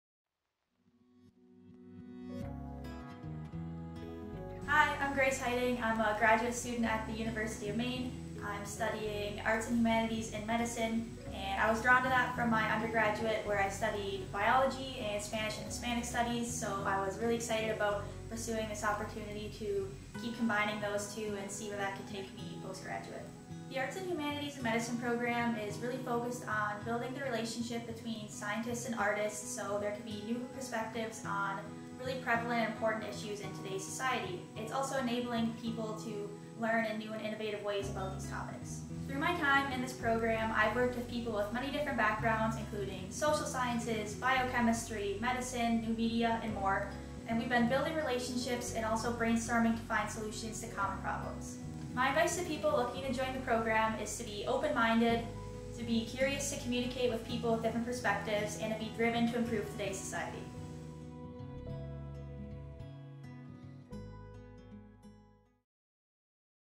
MaineArts & Humanities in Medicine - Grace Heiting
University of Maine Graduate Student Grace Heiting discusses the Arts & Humanities in Medicine program. She talks about her own experience and what to expect for anyone interested in joining the program.